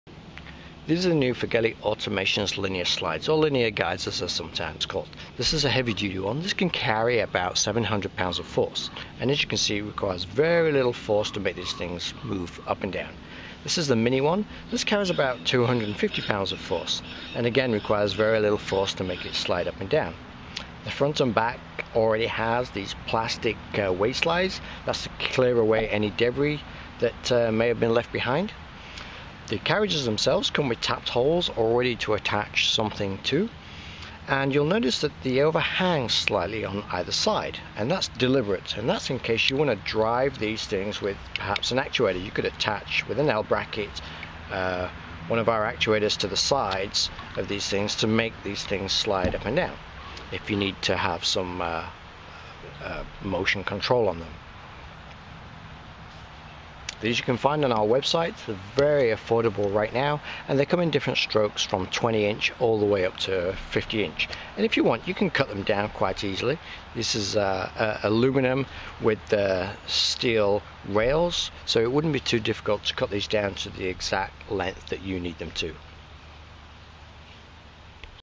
Linear Bearings 101 - What is a Linear Slide Bearing and how do they work.
Firgelli Automations Linear Bearings, can also be called Linear Guide rails or just Linear Slides or even Guide Rails, are Bearings that slide in a Linear Axis, so in a straight line.  Linear Bearing slide rails provide accurate, stable, and smooth linear guidance under a wide range of speeds, loads, conditions and space requirements.

Specifications:

1. Rail includes (1) Sliding Bearing Block (Additional sliding blocks can be purchased separately)
2. Standard length blocks and different Rail Lengths. Add Rails together to make very long Linear Bearings.
3. 750 lbs Max Loading per sliding block - add more sliders to reduce weight if required.
4. Temperature range -20 ~ 80℃
5. High load capabilities
6. Carbon steel rails as well and carbon steel bearings
7. Tapped mounting holes on the sliding block
8. Sliders come with re-circulating ball-bearing system so the smoothest glide possible
9. Frequent through holes for rail mounting.
10. Very affordable and versatile, ideal for many applications from Home Automation to Industrial Automation
11. Max Torque X, Y, Z directions: 1320 lb-inch
12. Weight 4.57 lbs. per foot